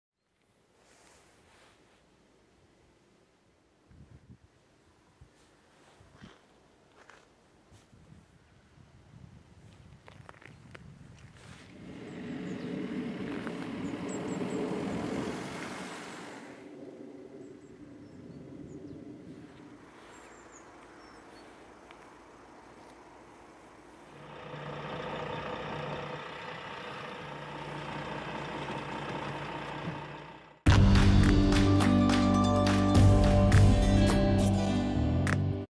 Automobiliste gewond na schuiver in Delden
Een automobiliste is gewond geraakt bij een ongeluk op de provinciale weg tussen Delden en Bornerbroek.Ze gleed met haar auto van de weg, vermoedelijk door gladheid.